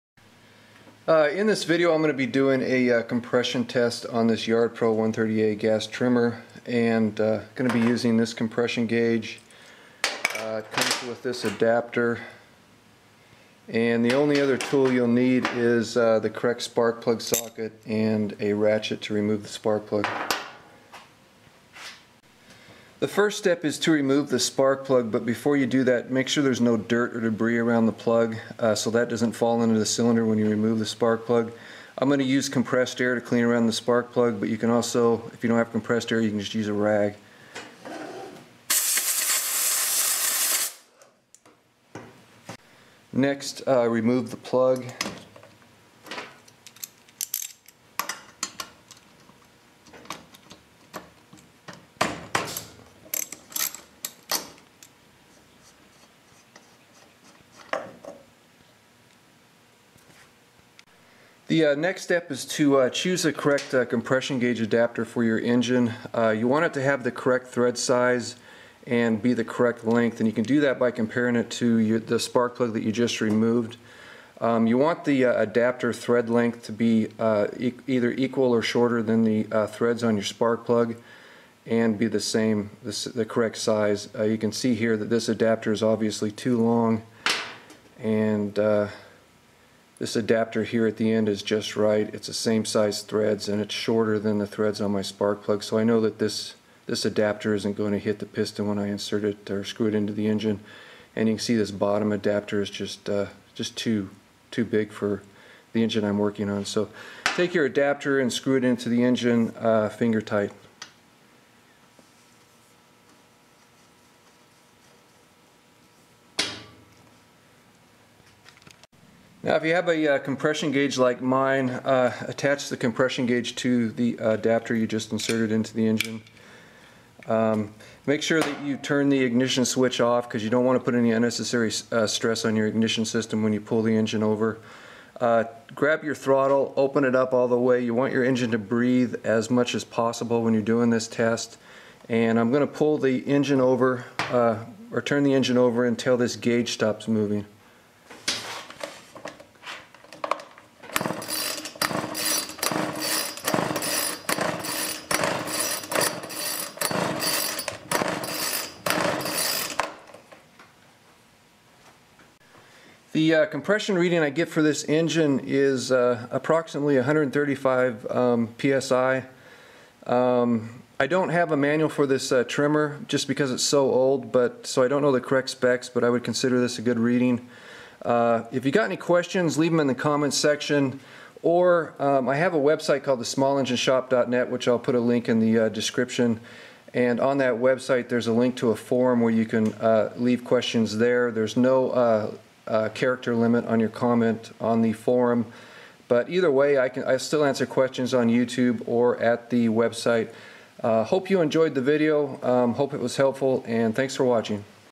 Compression Test on a 2 Stroke Trimmer
**Always follow the instructions in your repair manual when doing repair or maintenance work on Outdoor Power Equipment. Manuals can be found at the manufacturers website.**

In this video I do a compression test on a 2 stroke Yard Pro 130A.